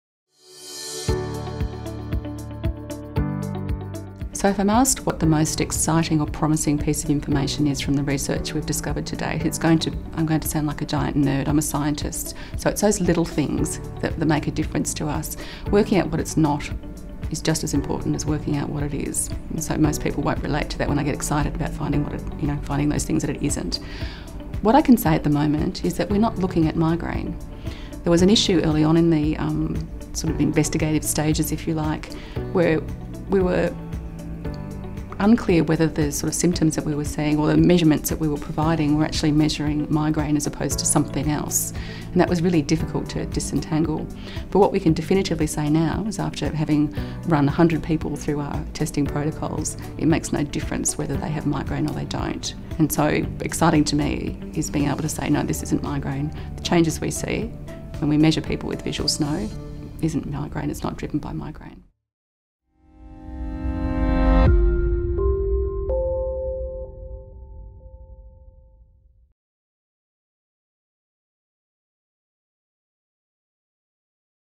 What is the most exciting or promising piece of information you discovered or learned about VSS?
You asked the questions, we have the answers! This series has given our experts the opportunity to share their latest findings on Visual Snow Syndrome research with you while also giving you a chance to ask the questions that have been on your mind.

In this video, Dr. Joanne Fielding answers the question: What is the most exciting or promising piece of information you discovered or learned about VSS?